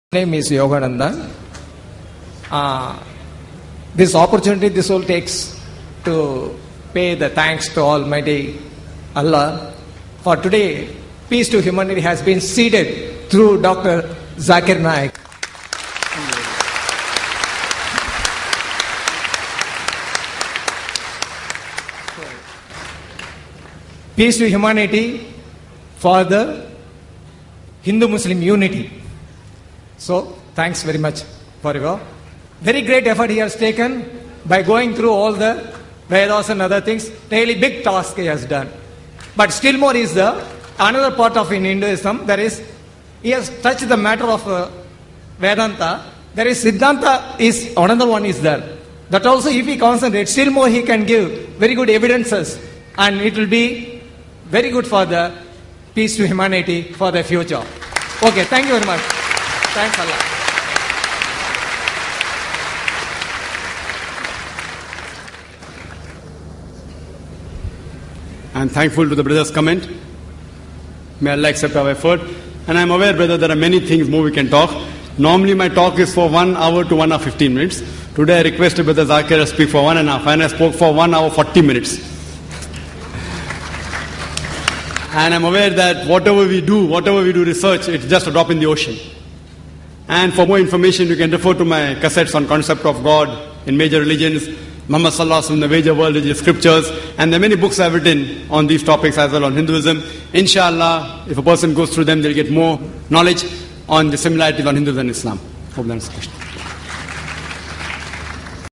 Pandangan Jujur Non-Muslim pada Dr. Zakir [Dr.Zakir Naik]
Rasa terima kasih pemuka Hindu pada Dr. Zakir Naik